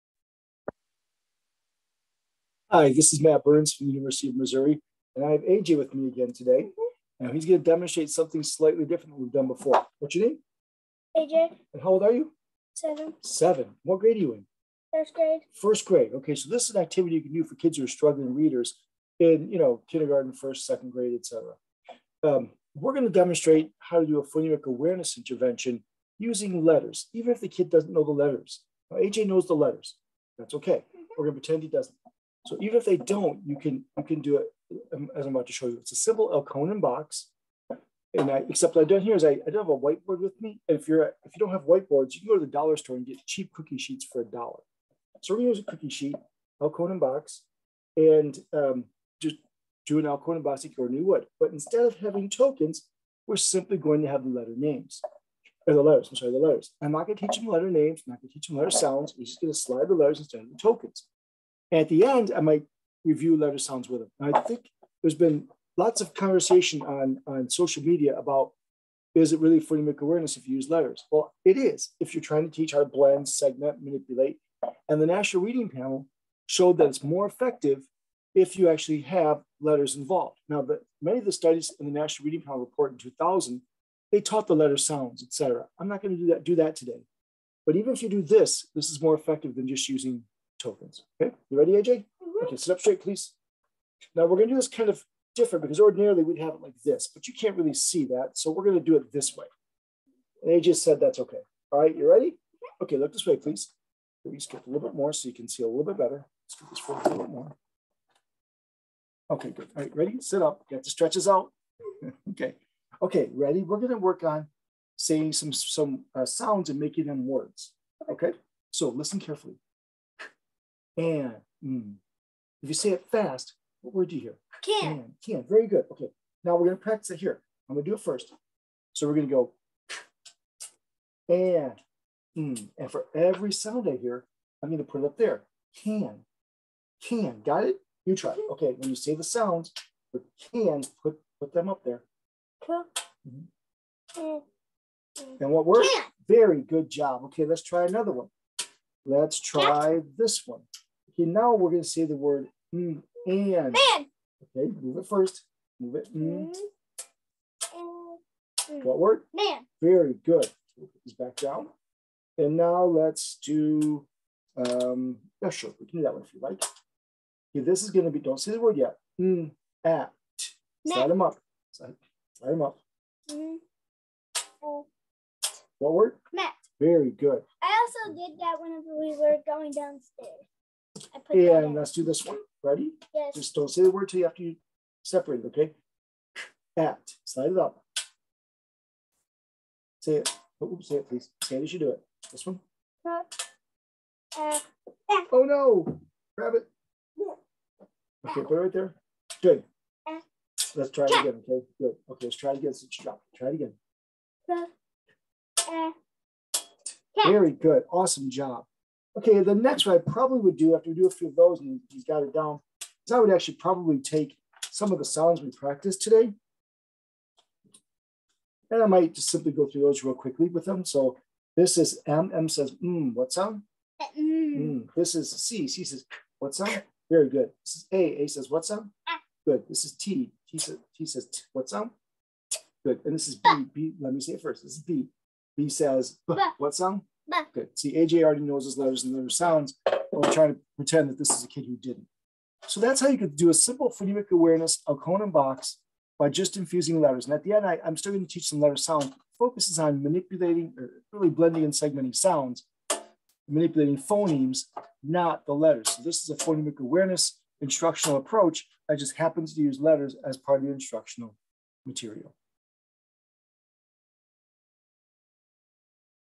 Phonemic Awareness with Letters
Phonemic awareness interventions are more effective if they have the students manipulate letters. Without letters = 0.38, 0.34 (NRP, 2000), 0.18 (Bus & IJzendoorn, 1999); With letters = 0.67, 0.61 (NRP, 2000), 0.66 (Bus & IJzendoorn, 1999). PA instruction is more effective if the students manipulate letters during it, but how do you do that if the kids do not know letters yet? Here's an example.